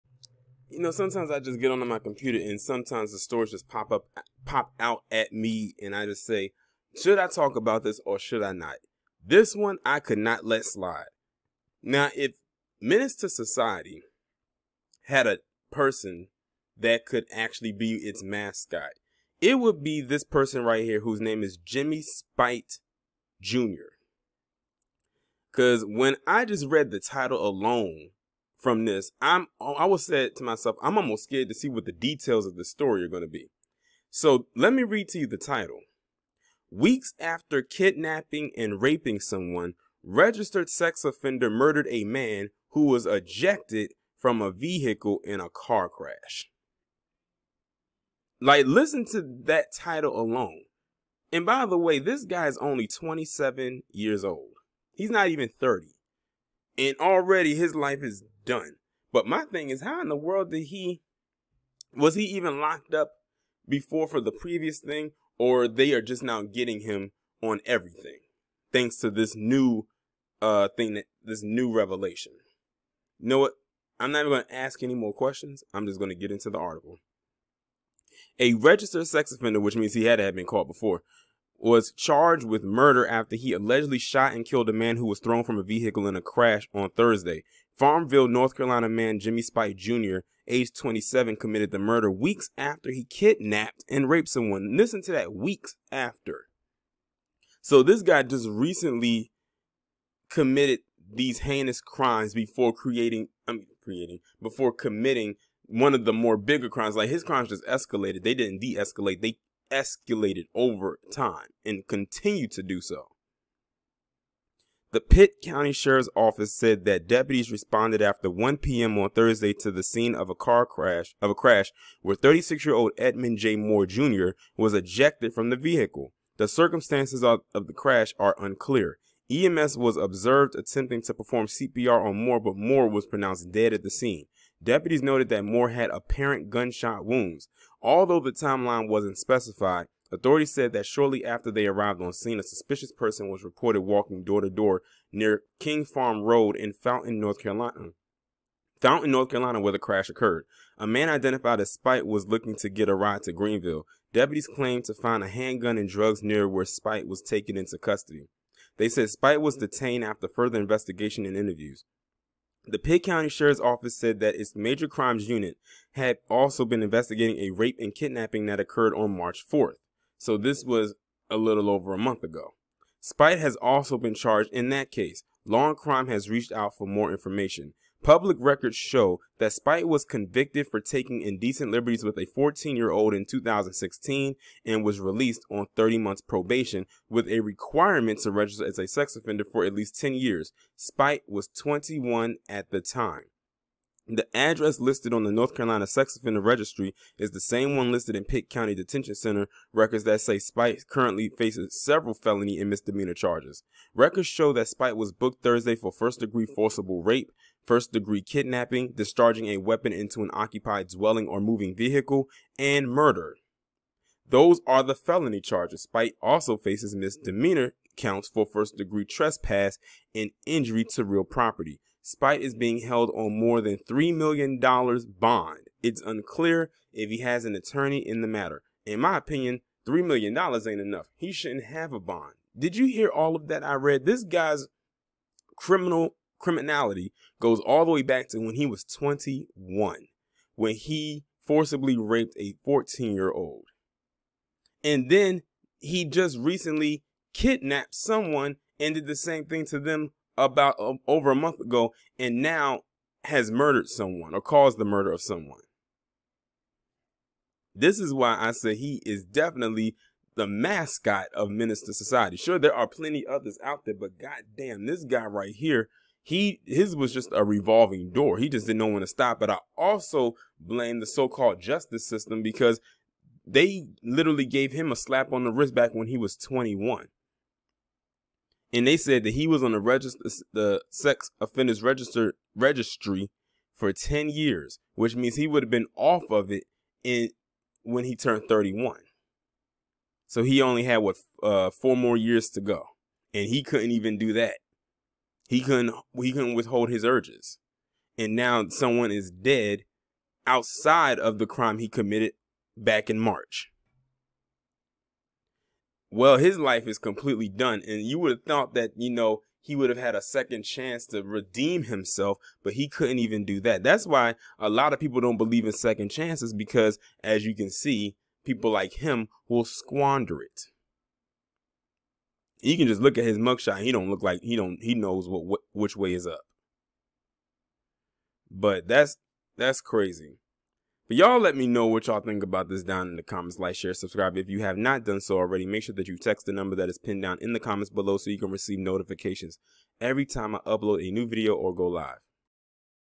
DNU: Jimmie Speight Jr
Jimmie Speight Jr. is a candidate for this edition of dumb negroes united because he has a rather interesting lengthy rap sheet that includes but not limited to kidnapping, rape, and now murder.